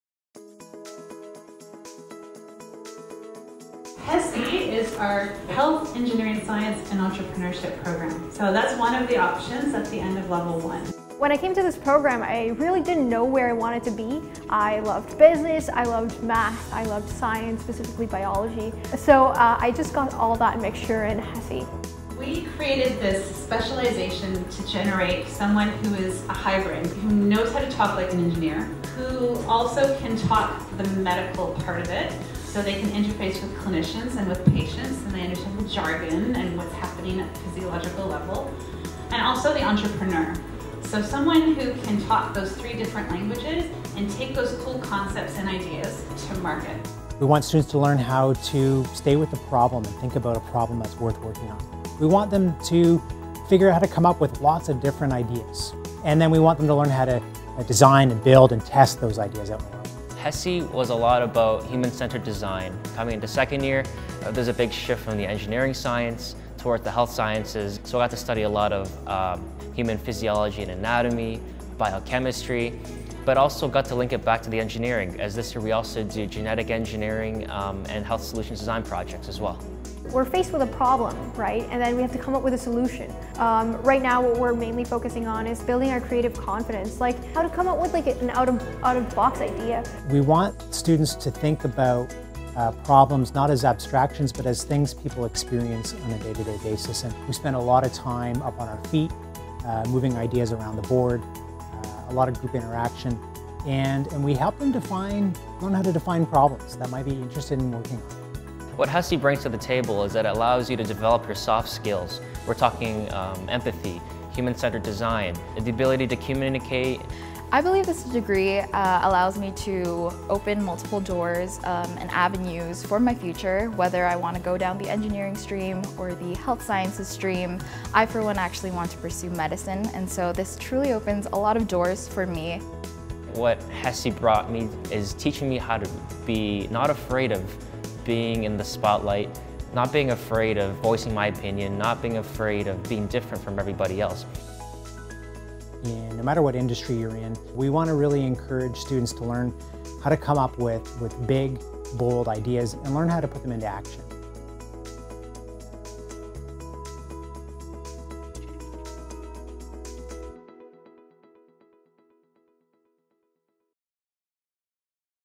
iBioMed - Health, Engineering Science and Entrepreneurship (HESE) Stream
iBioMed's HESE stream teaches students about leadership, finance, marketing, discovery, design thinking and more! Check out this video to learn where the HESE stream can take you!